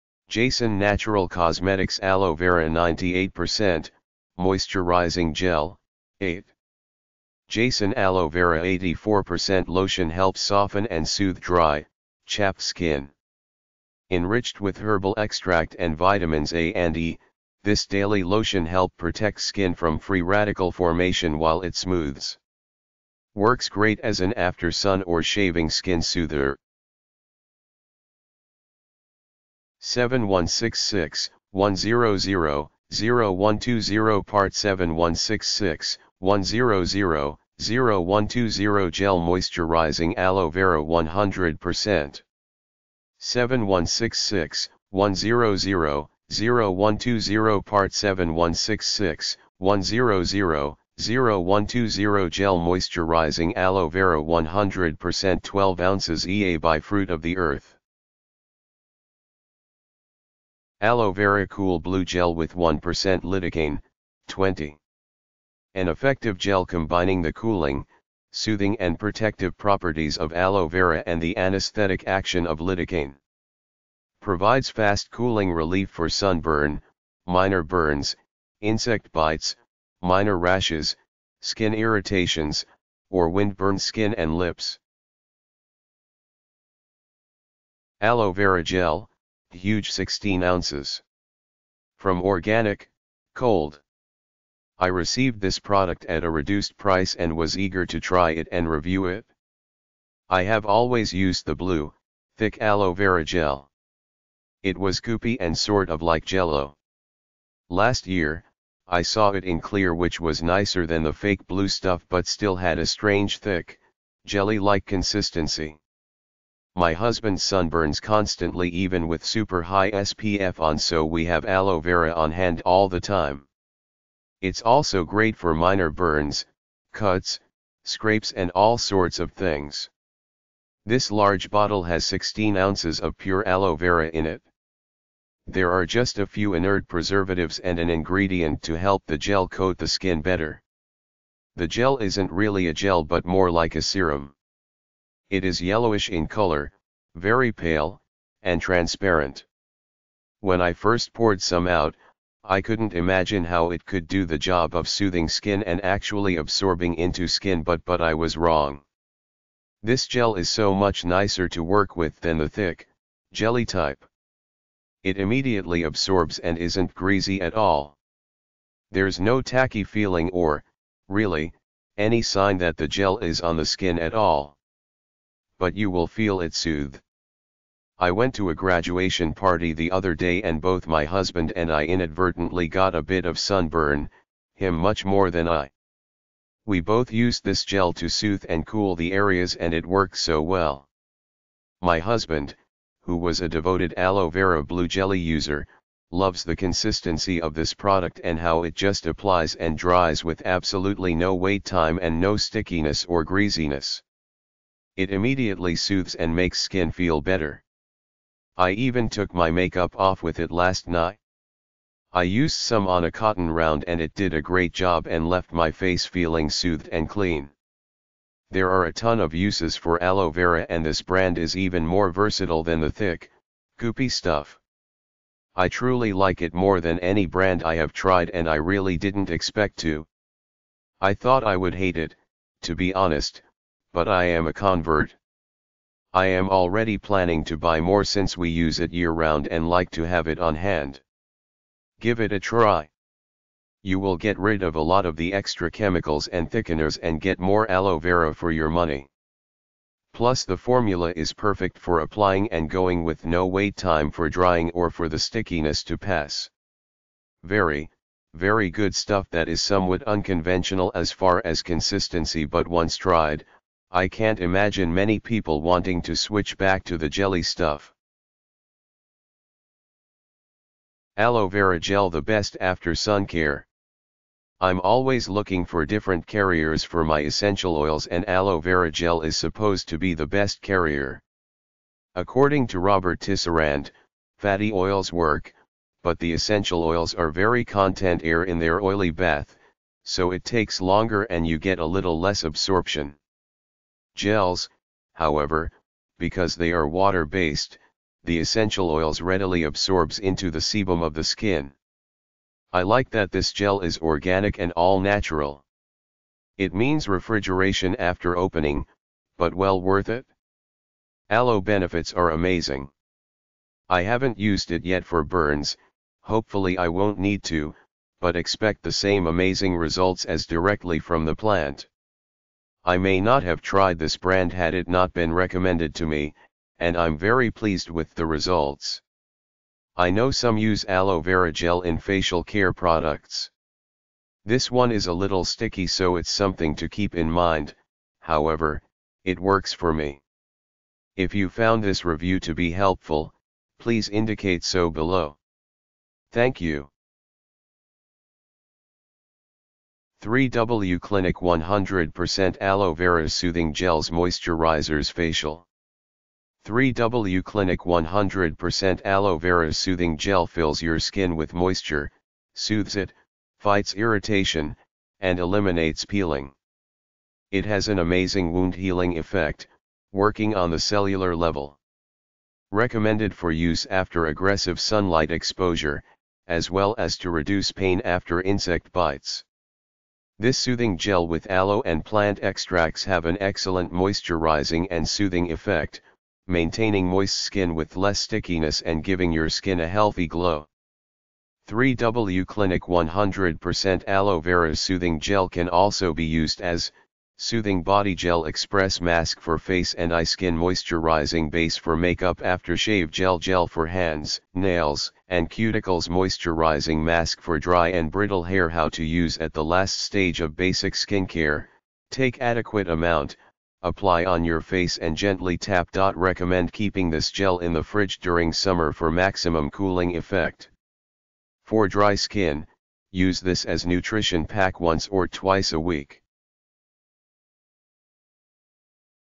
8 best aloe vera gel
1 - JASON Natural Cosmetics Aloe Vera 98%, Moisturizing Gel, 8 - JASON Aloe Vera 84% Lotion helps soften and soothe dry, chapped skin. Enriched with Herbal Extract and Vitamins A & E, this daily lotion help protect skin from free radical formation while it smoothes. Works great as an after sun or shaving skin soother.

2 - 100% Pure Aloe Vera Gel -- Without Chemicals and - Size: 100 ml the aloe vera gel is made from organically grown pure, green natural leaves without adding absolutely no chemicals or additives. It is viscous and colorless, and is soluble in water and other organic solvents. The raw gel is cleansed with rose water to remove raw smell which is necessary.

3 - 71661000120 Part# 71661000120 - Gel Moisturizing Aloe Vera 100% - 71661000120 Part# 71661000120 - Gel Moisturizing Aloe Vera 100% 12oz Ea By Fruit of the Earth

4 - Aloe Vera Cool Blue Gel With 1% Lidocaine, 20 - An effective gel combining the cooling, soothing and protective properties of Aloe Vera and the anesthetic action of Lidocaine.  Provides fast cooling relief for sunburn, minor burns, insect bites, minor rashes, skin irritations, or windburned skin and lips.

5 - Aloe Vera Gel, HUGE 16 Oz. From Organic, Cold - I received this product at a reduced price and was eager to try it and review it. I have always used the blue, thick Aloe Vera gel. It was goopy and sort of like Jello. Last year, I saw it in clear which was nicer than the fake blue stuff but still had a strange thick, jelly-like consistency.

6 - Aloe Vera Gel - The Best After Sun Care - I'm always looking for different carriers for my essential oils and aloe vera gel is supposed to be the best carrier! According to Robert Tisserand, fatty oils work, but the essential oils are very content air in their oily bath, so it takes longer and you get a little less absorption.

7 - 3w Clinic 100% Aloe Vera Soothing Gels Moisturizers Facial - 3W Clinic 100% Aloe Vera Soothing Gel fills your skin with moisture, soothes it, fights irritation, and eliminates peeling. It has an amazing wound-healing effect, working on the cellular level. Recommended for use after aggressive sunlight exposure, as well as to reduce pain after insect bites. This soothing gel with aloe and plant extracts have an excellent moisturizing and soothing effect, maintaining moist skin with less stickiness and giving your skin a healthy glow.

8 - Aloe Vera Gel 2 Pack - 99.75% Pure, Cold-Pressed, - This is the most pure aloe I've found. Many people are surprised by its less jelly like consistency, and don't like that. What they don't realize is that other products get that viscous quality with additives. I love this for its purity. It has been the best for me.